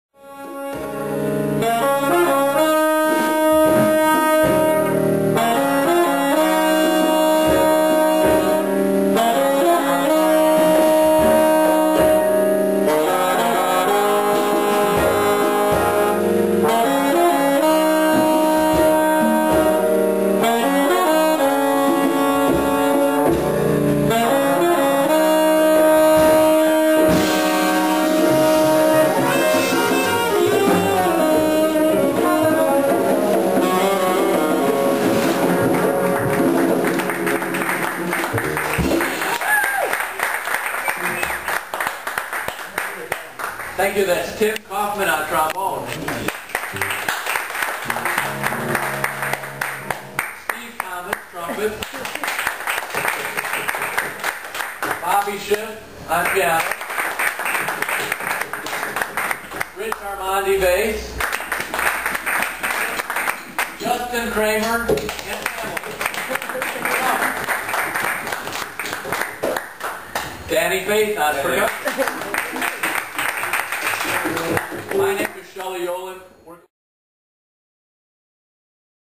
Live Jazz on Lincoln Avenue
Live Jazz on Lincoln Avenue with Shelley Yoelin's Jazz Workshop! 5155 N. Lincoln Avenue, Chicago, IL!